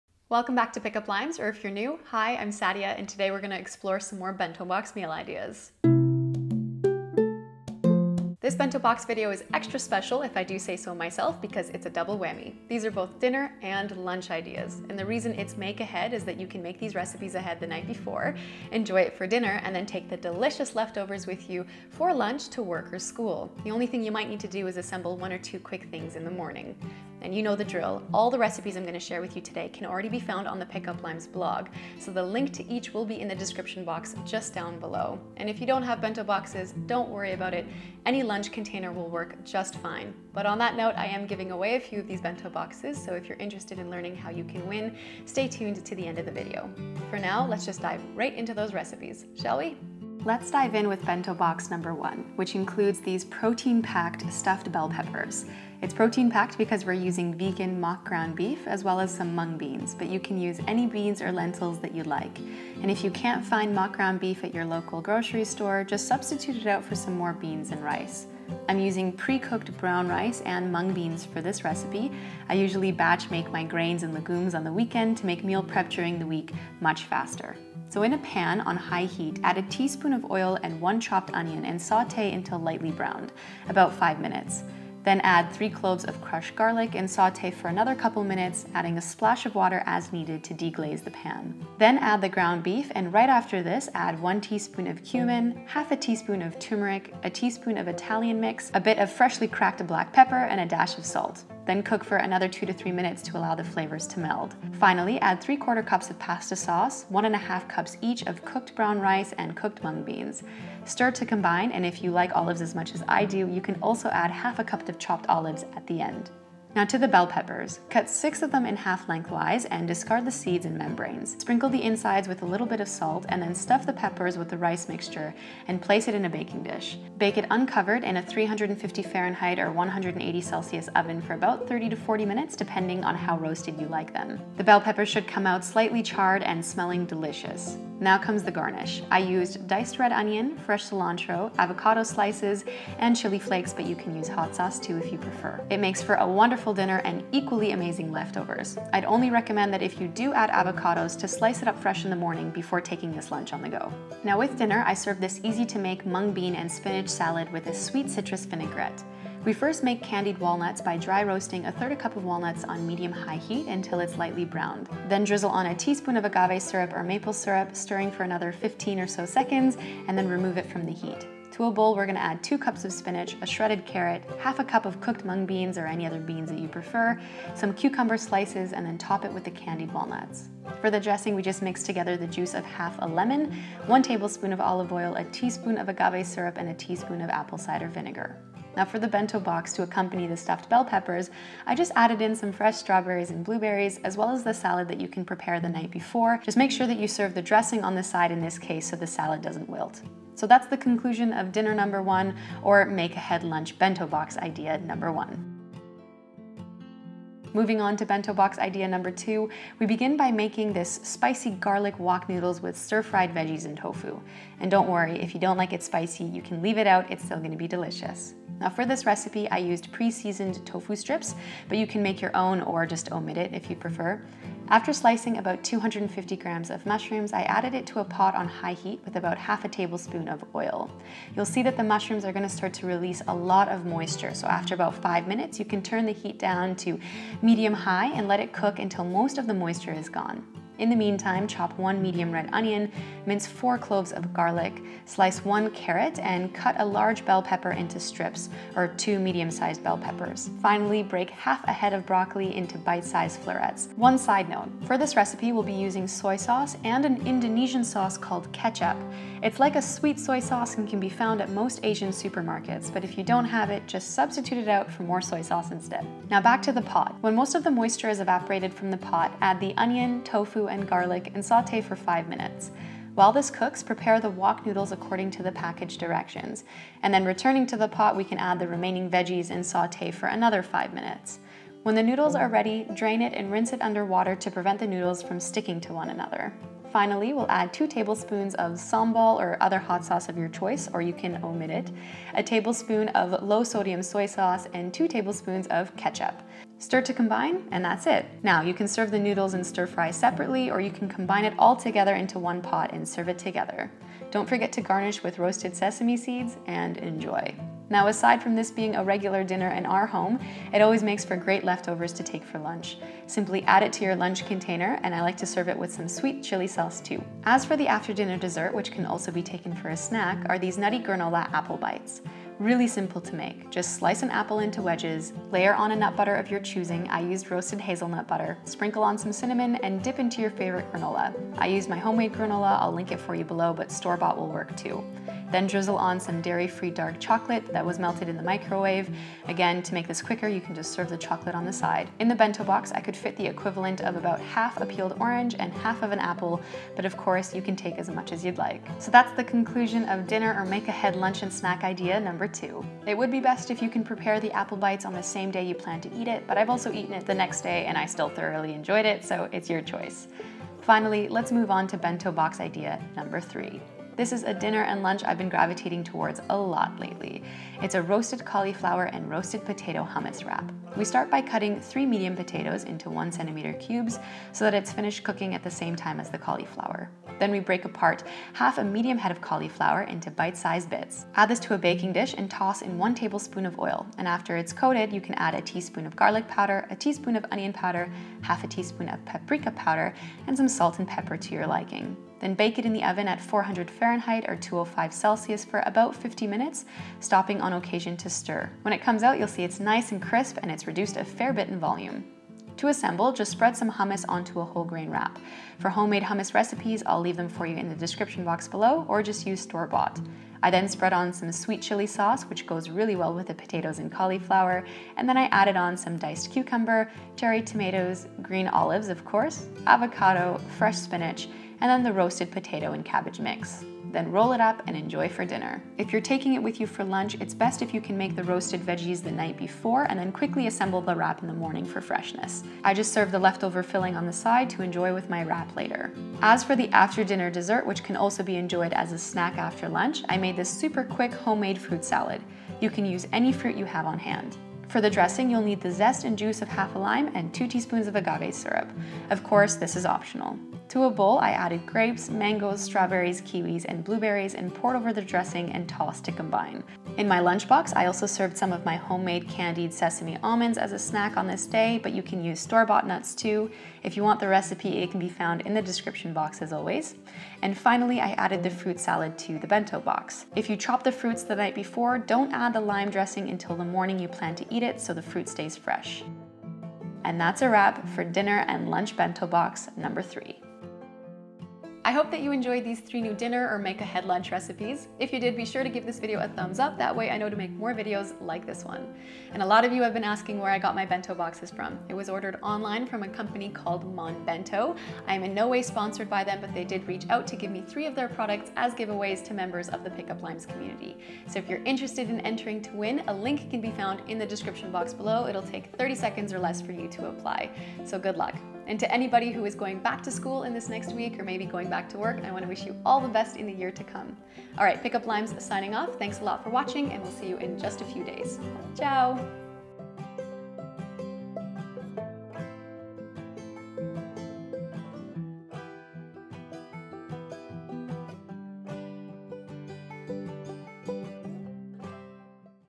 MAKE-AHEAD VEGAN LUNCH IDEAS » bento box
📌 The giveaway is now CLOSED but you can get your own bento box

RECIPES
00:00 Intro
00:49 Stuffed bell peppers
03:27 Spicy garlic wok noodles
05:17 Granola apple bites
05:32 Crunchy pecan & cranberry granola
06:00 Roasted cauliflower & potato wraps
07:17 Summer fruit salad
07:37 Candied sesame almonds
08:01 Outro

WHAT WE USE @ THE PUL KITCHEN (2019)
» Food processor
» Hand blender:
» Cast iron pot:
» Mini spice jars:
» Air-tight containers:

❤ Sadia